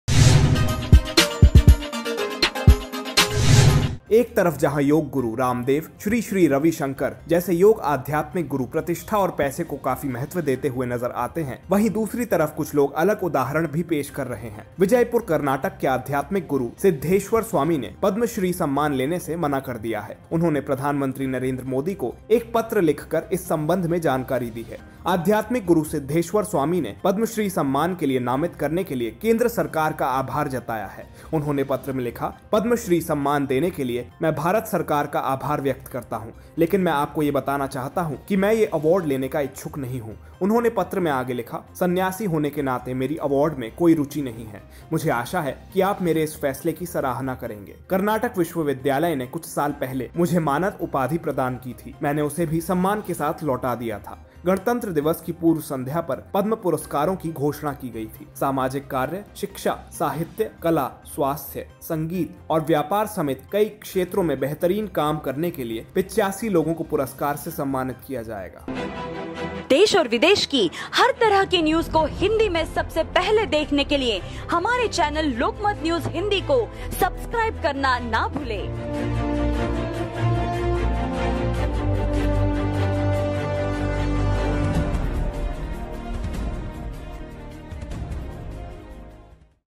जानिए क्यों इन आध्यात्मिक गुरु ने पद्मश्री लेने से इंकार कर दिया | Lokmat Hindi News
एक तरफ जहां योग गुरु रामदेव, श्री श्री रविशंकर जैसे योग-आध्‍यात्‍मिक गुरु प्रतिष्ठा और पैसे को काफी महत्व देते हुए नजर आते हैं, वहीं दूसरी तरफ कुछ लोग अलग उदाहरण भी पेश कर रहे हैं। विजयपुर, कर्नाटक के आध्यात्मिक गुरु सिद्धेश्वर स्वामी ने पद्मश्री सम्मान लेने से मना कर दिया है। उन्होंने प्रधानमंत्री नरेंद्र मोदी को एक पत्र लिख कर इस संबंध में जानकारी दी है।आध्यात्मिक गुरु सिद्धेश्वर स्वामी ने पद्मश्री सम्मान के लिए नामित करने के लिए केंद्र सरकार का आभार जताया है। उन्होंने पत्र में लिखा, "पद्मश्री सम्मान देने के लिए मैं भारत सरकार का आभार व्यक्त करता हूं। लेकिन मैं आपको यह बताना चाहता हूं कि मैं यह अवॉर्ड लेने का इच्छुक नहीं हूं।'उन्होंने पत्र में आगे लिखा, "संन्यासी होने के नाते मेरी अवॉर्ड में कोई रूचि नहीं है। मुझे आशा है कि आप मेरे इस फैसले की सराहना करेंगे। कनार्टक विश्वविद्यालय ने कुछ साल पहले मुझे मानद उपाधि प्रदान की थी। मैंने उसे भी सम्मान के साथ लौटा दिया था।"गणतंत्र दिवस की पूर्व संध्‍या पर पद्म पुरस्‍कारों की घोषणा की गई थी। सामाजिक कार्य, शिक्षा, साहित्य, कला, स्वास्थ्य, संगीत और व्यापार समेत कई क्षेत्रों में बेहतर काम करने के लिए 85 लोगों को पुरस्कार से सम्मानित किया जाएगा